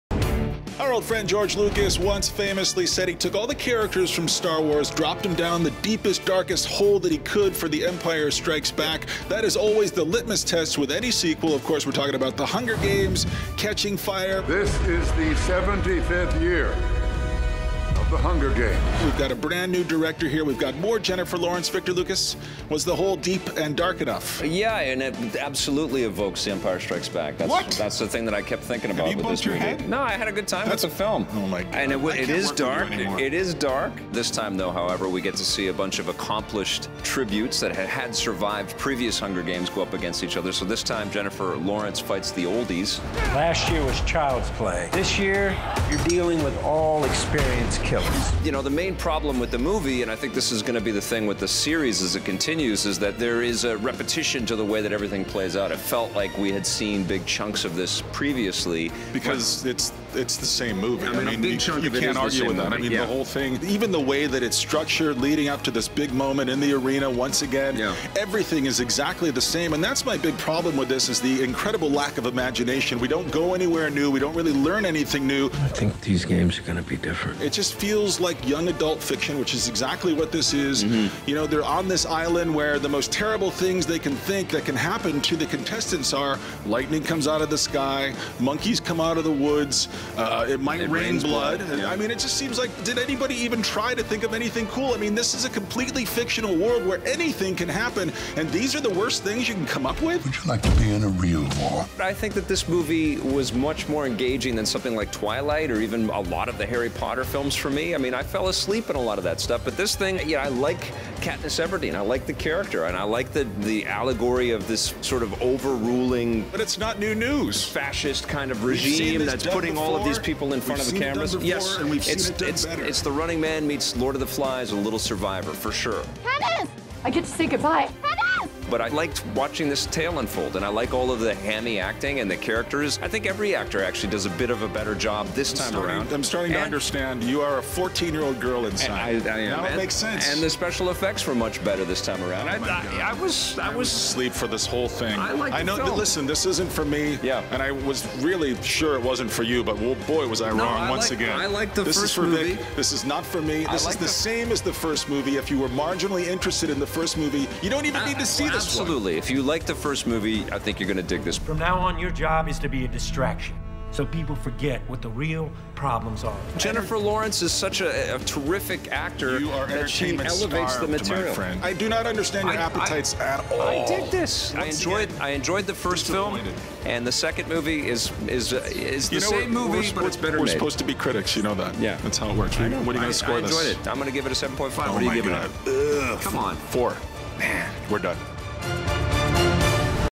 The Hunger Games: Catching Fire Review
Victor and Scott head back into The Hunger Games with Catching Fire.